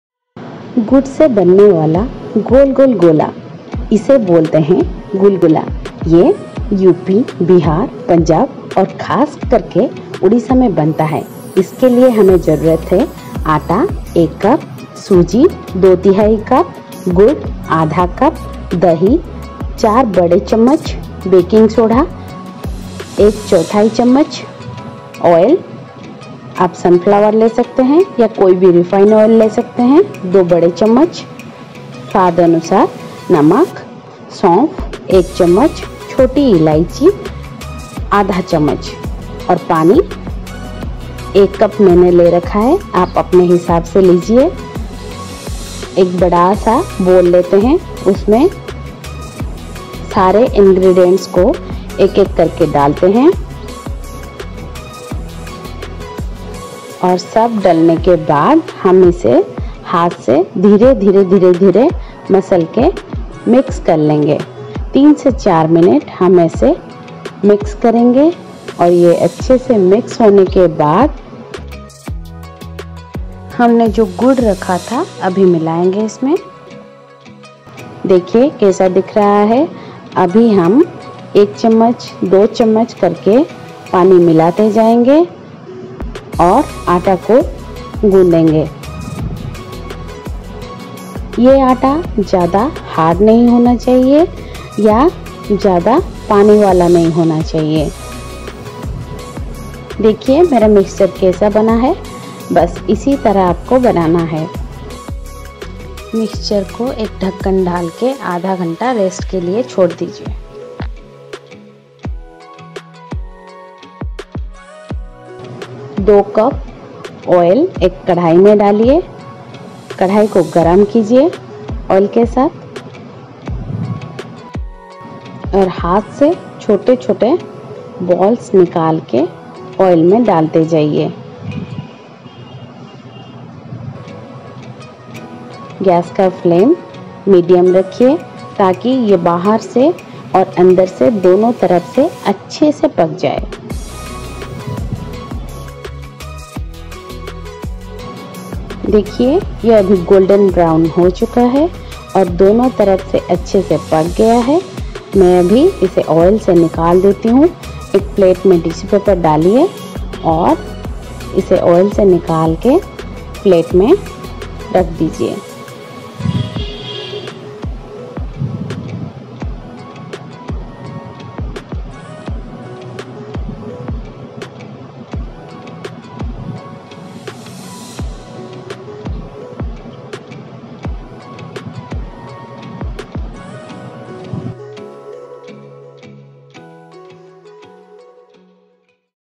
Gulgula | ଗୁଲୁଗୁଲା | gulgulle | khasta gulgula | odia recipe gulgula
today we will make gulgulla. This recipe is a quick and easy recipe. Most of the people love to eat this with tea or aloomatar curry.

Rava / semolina 1/4cup

Powdered jaggery 1/4cup

Crushed cardamom 2
curd 2tbsp

Oil/ghee for deep frying